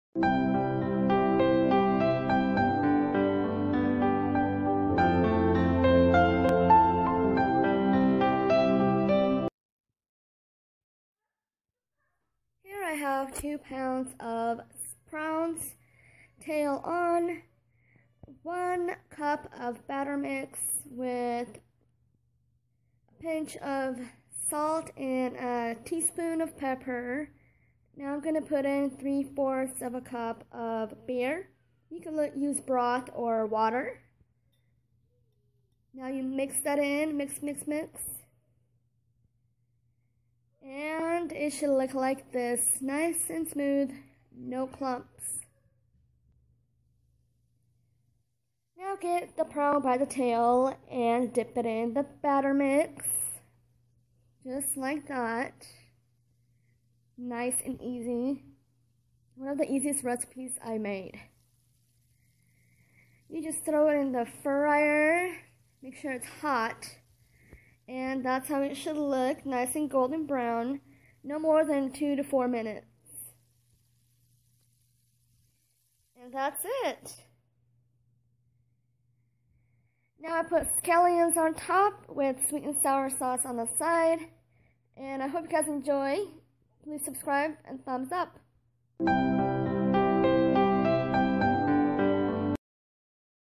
Beer batter fried prawns.
This recipe took me 15 to 20 minutes to cook 2 pounds prawns. Lovte this recipe for movie night or game days . Very easy and fast to prepare.